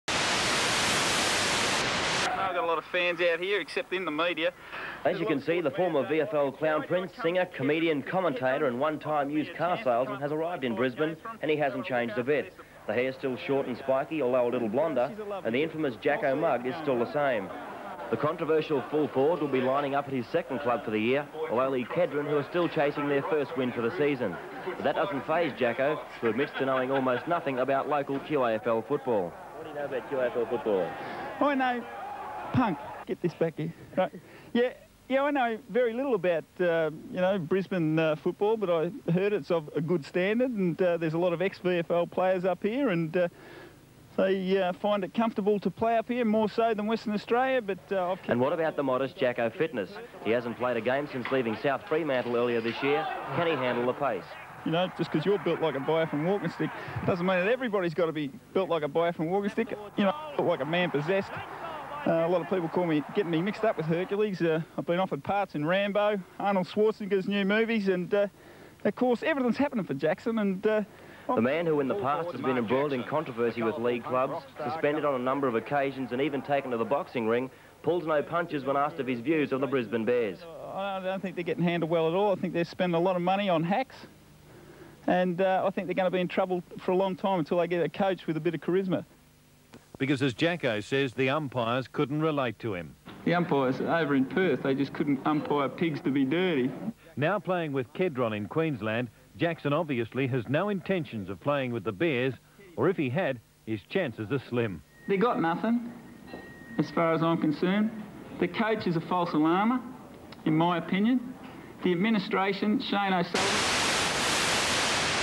Mark Jacko Jackson slams Brisbane Bears
In 1987 Jacko played for Kedron in the QAFL. In these two news clips Jacko talks up the news and slags off the administration of the newly formed Brisbane Bears. Turns out he was pretty spot on.